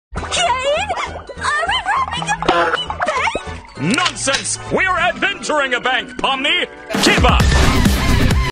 caine and pomni rob a bank in 3D
it's payday fellas

Caine and Pomni rig Release by EnlargedQuack